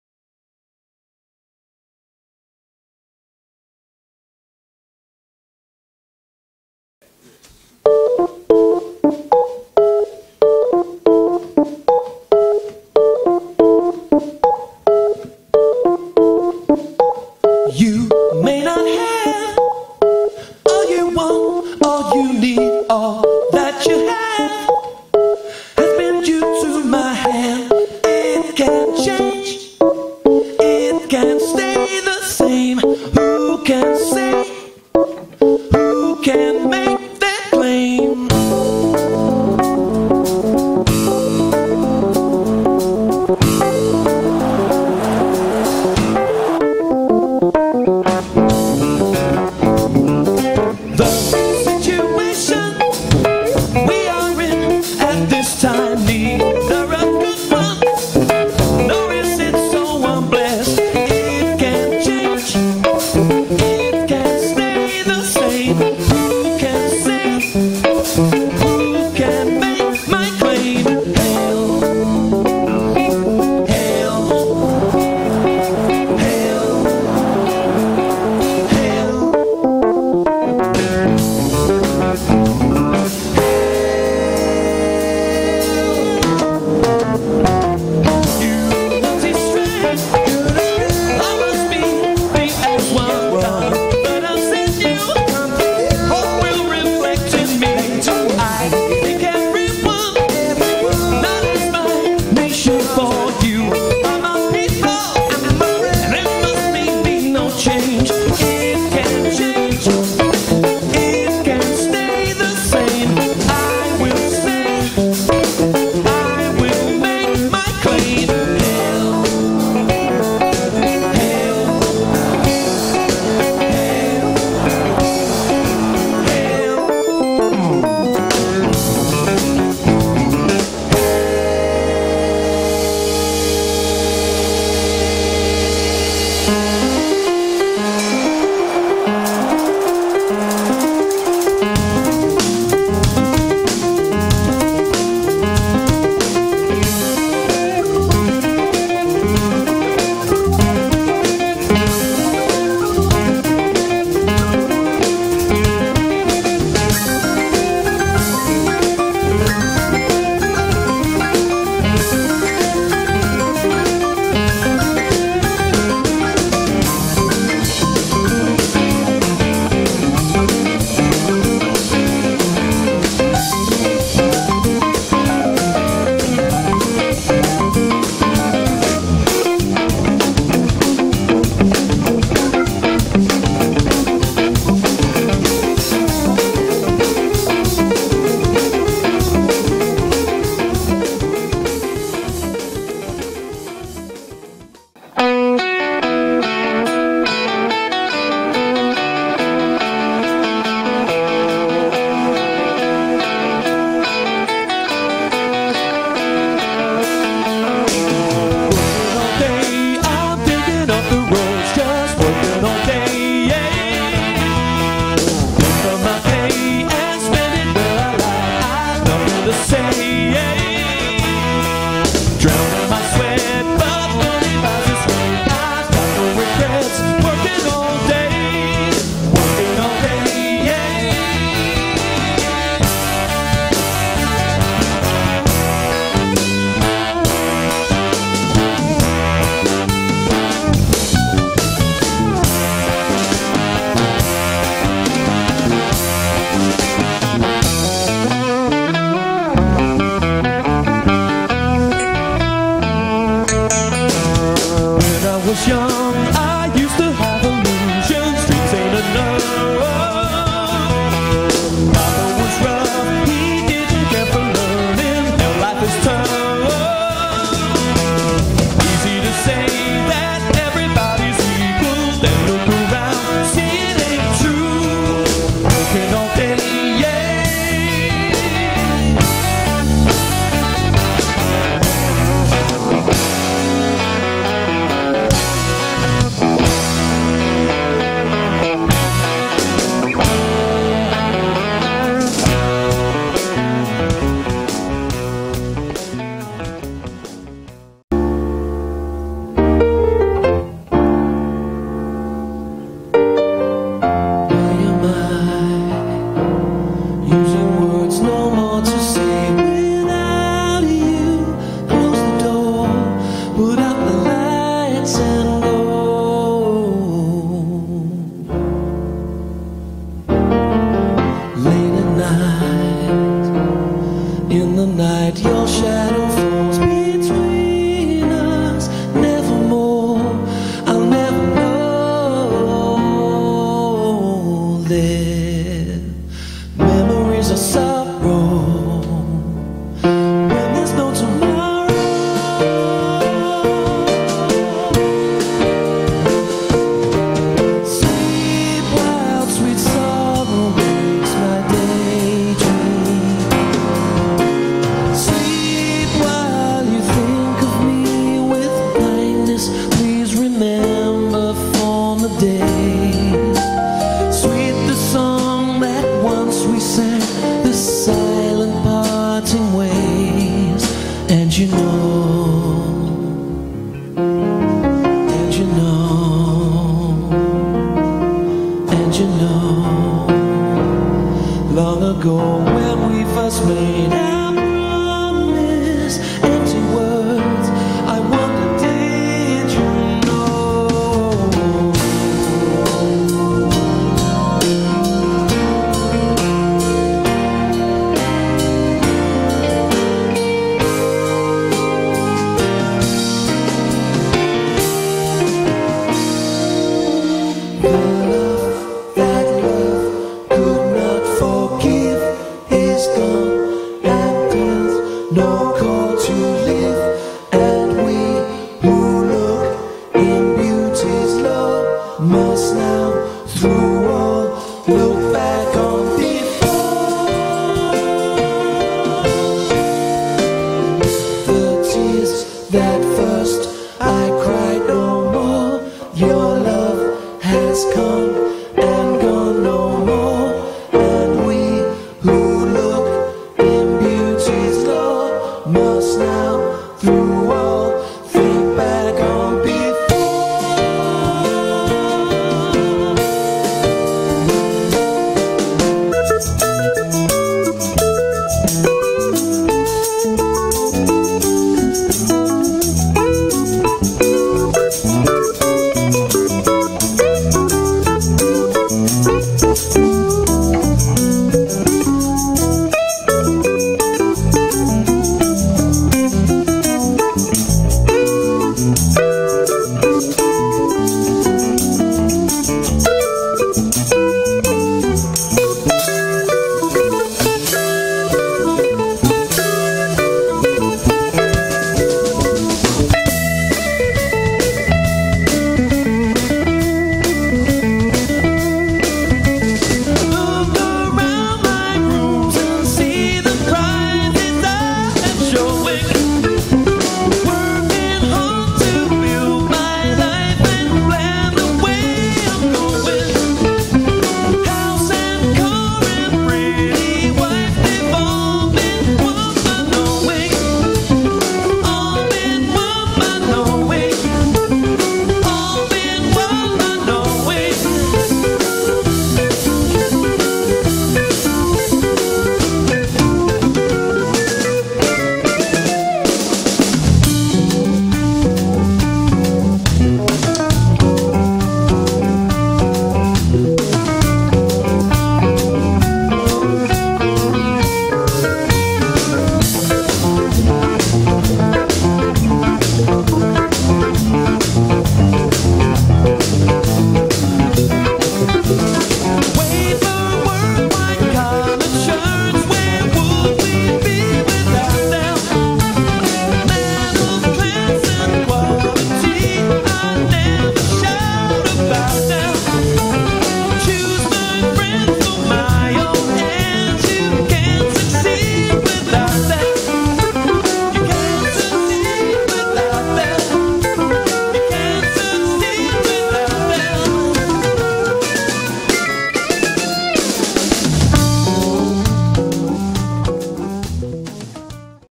Three Friends - In Tokyo
Four snippets of Three Friends playing a club in Tokyo December 2010. The vid and audio was shot by a Small Handy Cam in the audience.